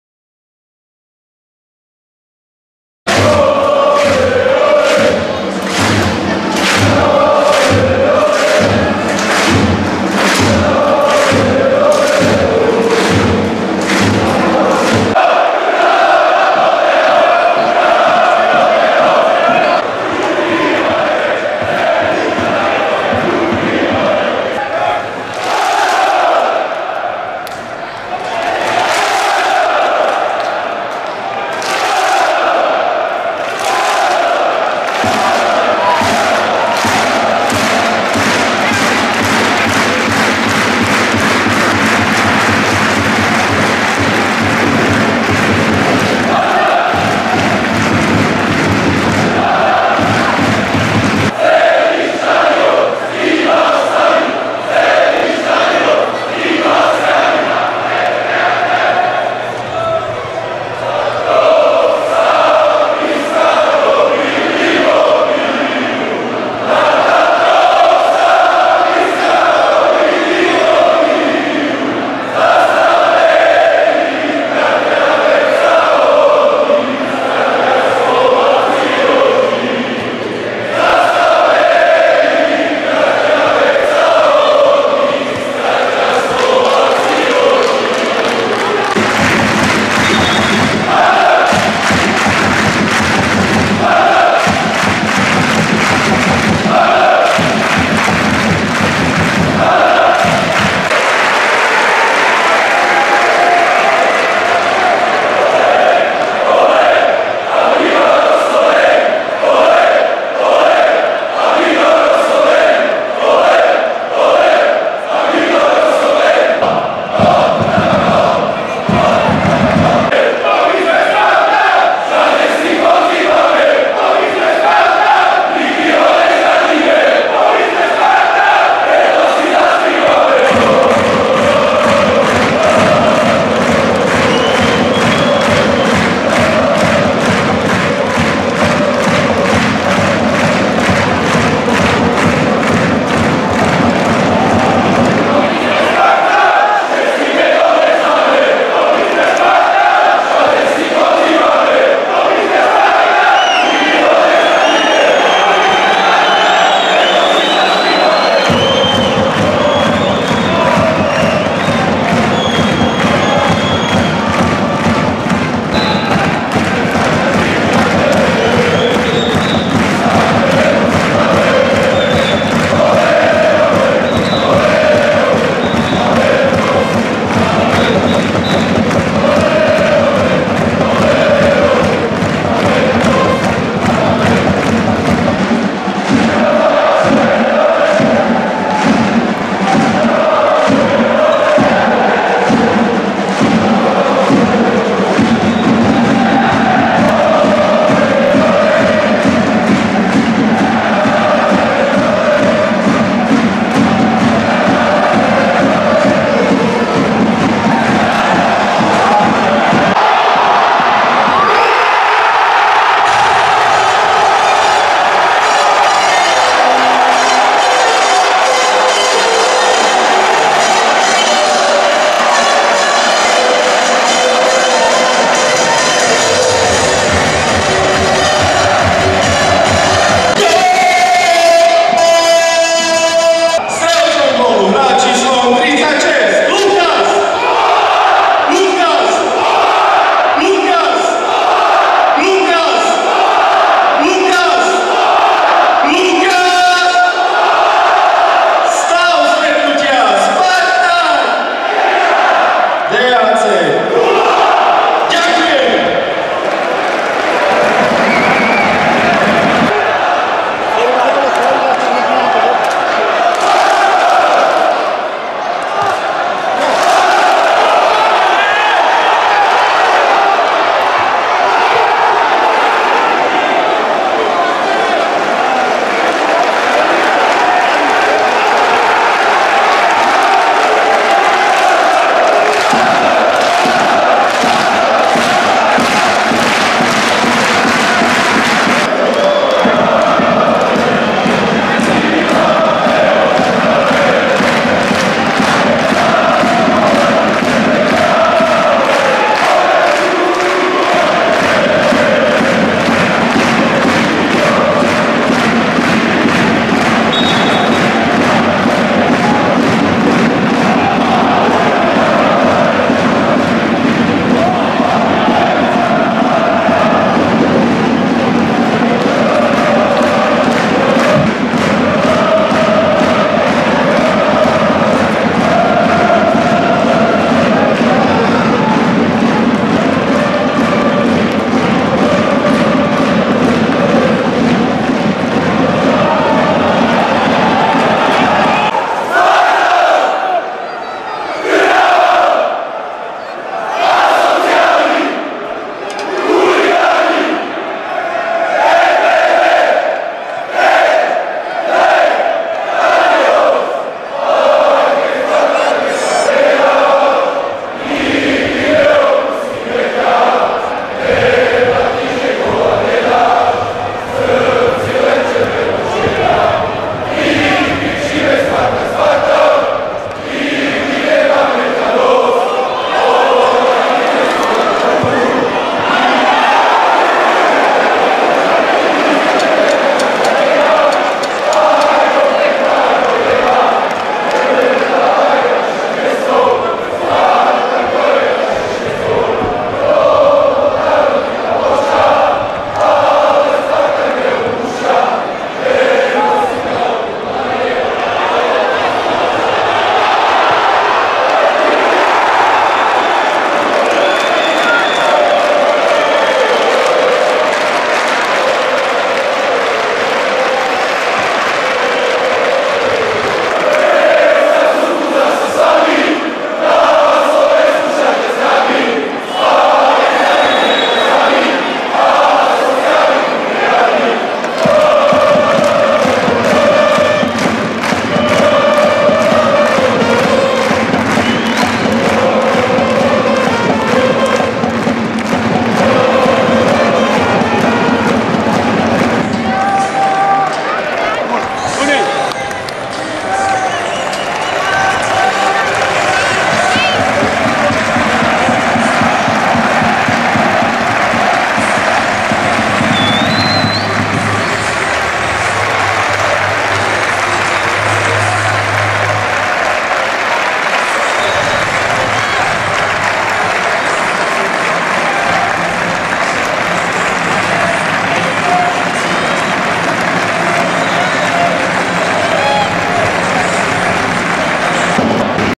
20191005 FCST vs DAC1904 part II
Spartak Trnava – Dunajská Streda 1:2 (0:1)
Goals: 68. Lovat -  5. A. Fábry, 74. K. Vida
YC: Mesík - E. Ramirez, Jedlička
Referees: Kružliak – Somoláni, Hancko
Attendance: 3 949
SPARTAK: Rusov – Turňa (79. Yilmaz), Menich, Mesík – Diogo, Halilovič (79. Grendel), Gamboš (66. Dangubič), Tešija, Lovat – Sobzcyk, Tavares. Coach: Chéu.
DAC: Jedlička – Blackman, Kružliak, Beskorovajnyj, Davis – Oravec – A. Fábry (68. Divkovič), Ronan, M. Vida, K. Vida – E. Ramirez. Coach: Hyballa.